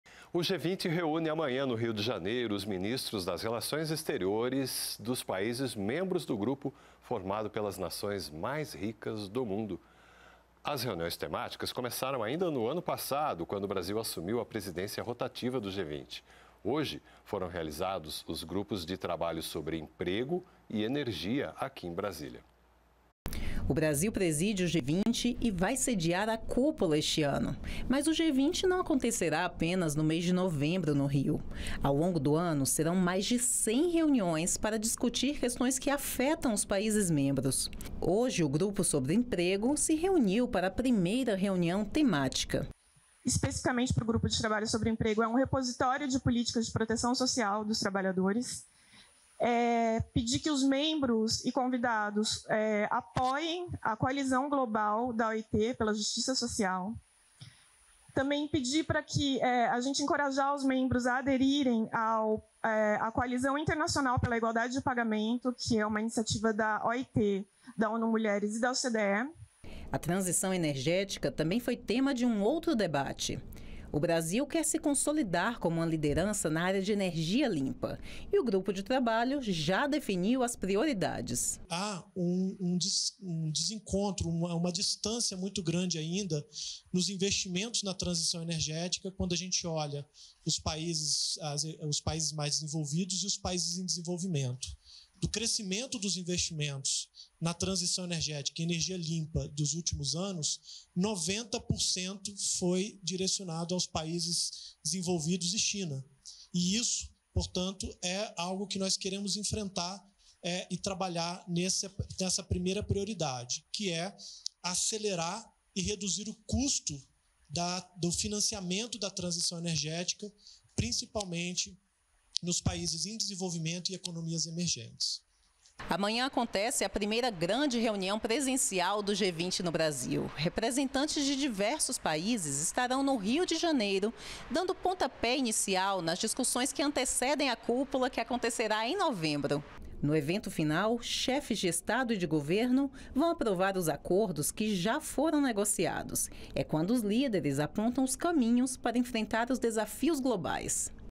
G20 REÚNE MINISTROS DE RELAÇÕES EXTERIORES NO RIO DE JANEIRO
O G20 reúne nesta quarta (21), no Rio de Janeiro, os ministros de Relações Exteriores dos países membros. As reuniões temáticas começaram ainda em 2023, quando o Brasil assumiu a presidência rotativa do grupo formado pelas nações mais ricas do mundo.